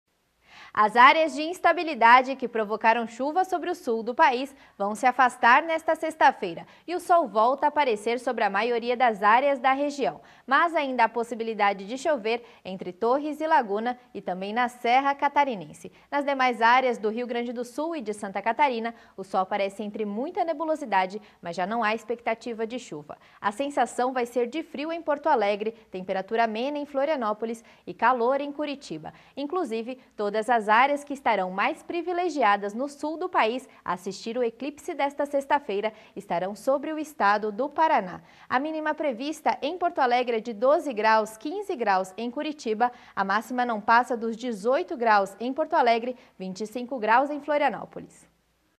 Nesta sexta-feira a chuva para e o sol volta aparecer no Sul do país, atenção produtores de fumo!
Curta
Compartilhe

Imagens:Clima tempo